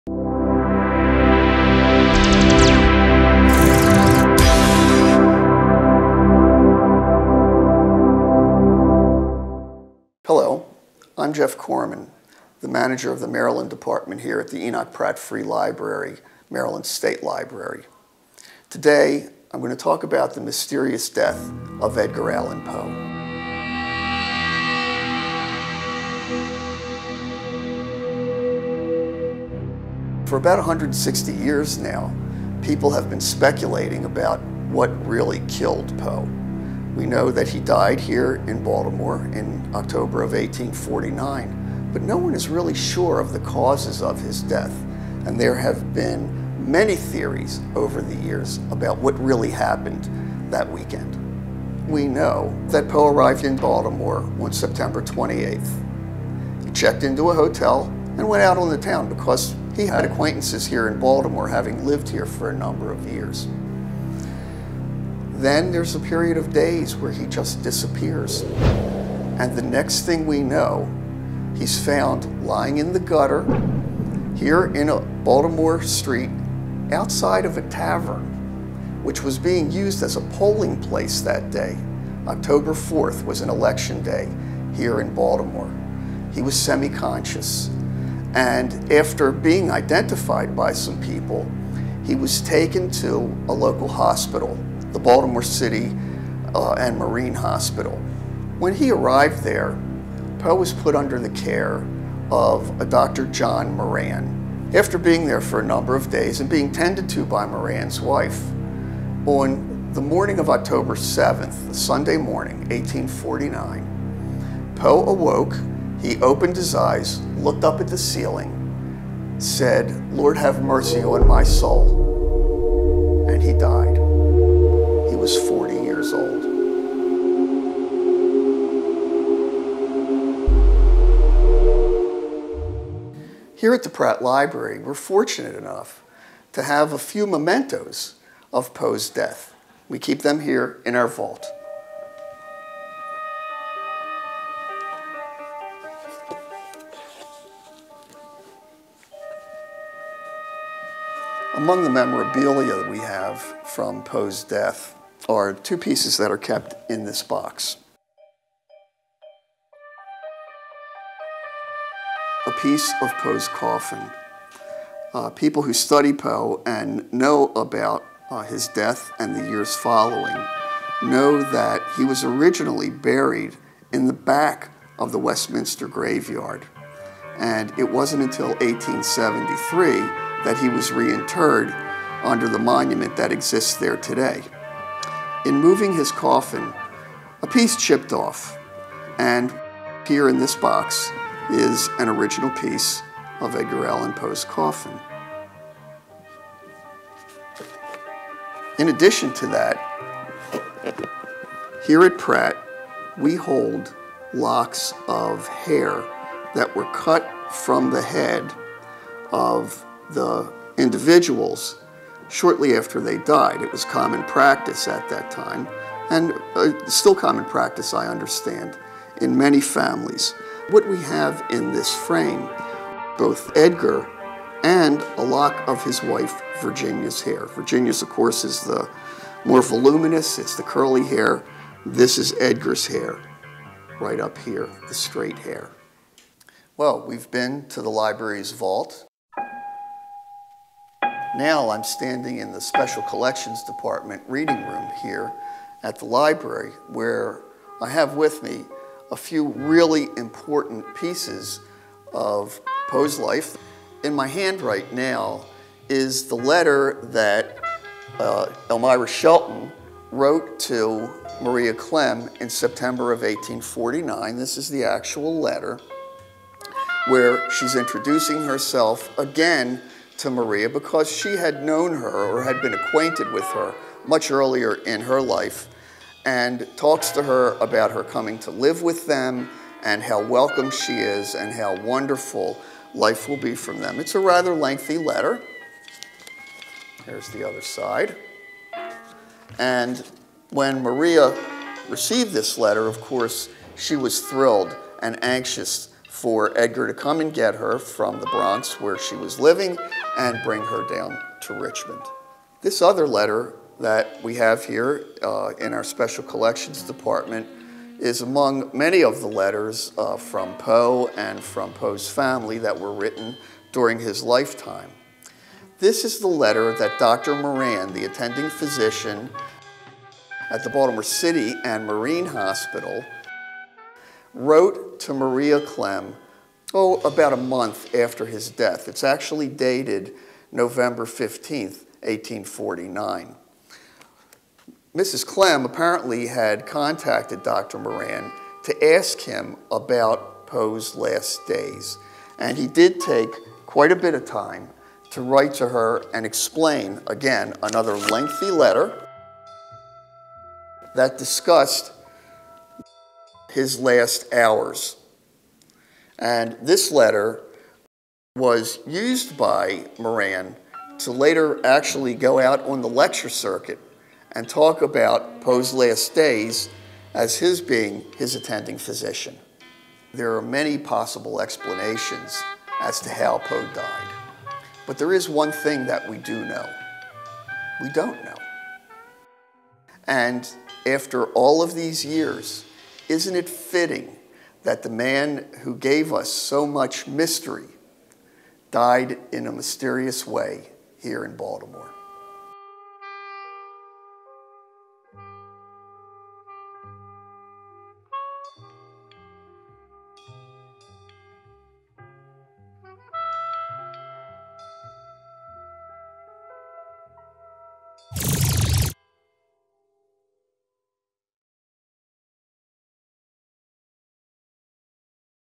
Extended Edition of Jeff Korman Presents: The Last Days of Poe
Jeff Korman reveals the hidden treasures of Edgar Allan Poe the Pratt keeps safe in its vault and Special Collections. Jeff is the Manager of The Maryland Department at The Enoch Pratt Free Library and Maryland State Library Resource Center, located in Baltimore, Maryland.